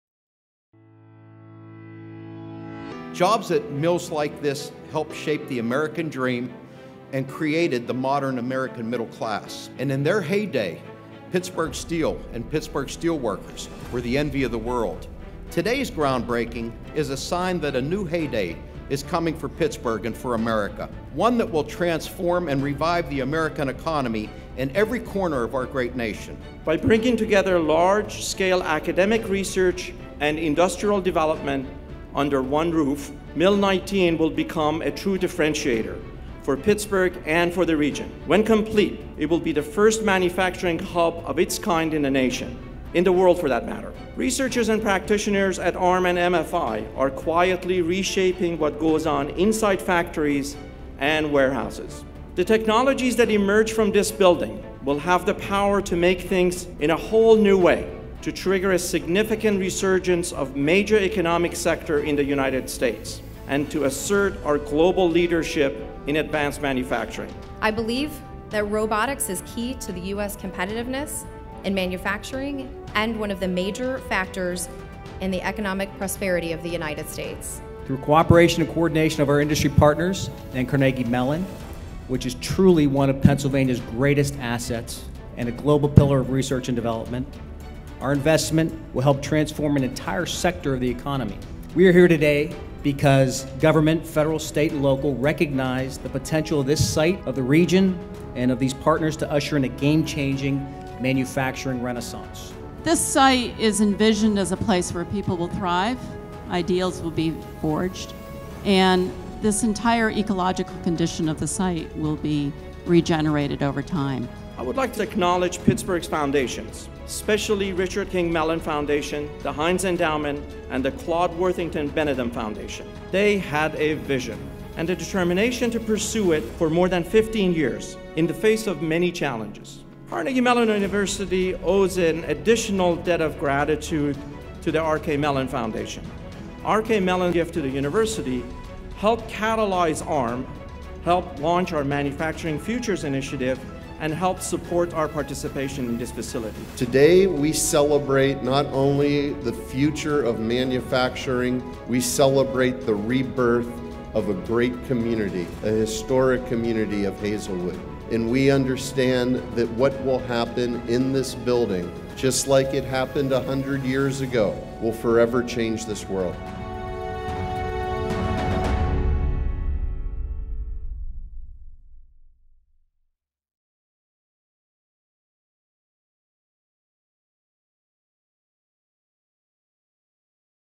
The Future of Manufacturing Starts Today
On November 20, 2017, Carnegie Mellon University and the Advanced Robotics for Manufacturing Institute (ARM) broke ground on a new hub for manufacturing technology on the iconic Hazelwood Green site, entering a new chapter in Pittsburgh’s resurgence as a global leader in innovation.

Highlights from the event included additive and robotic manufacturing demonstrations and remarks from speakers including U.S. Rep. Mike Doyle, CMU's Interim President Farnam Jahanian, Director of Manufacturing Institutes for the Department of Defense Tracy Frost, Pennsylvania Secretary of Community and Economic Development Dennis Davin, Hazelwood Green Project Director Rebecca Flora and Pittsburgh Mayor Bill Peduto. The event culminated with participants signing a steel sculpture that will be placed in the lobby of the new building, aided by special robot guest Igor of HEBI Robotics. During the sculpture signing, a large banner featuring one of the renderings of the new building rose in the background.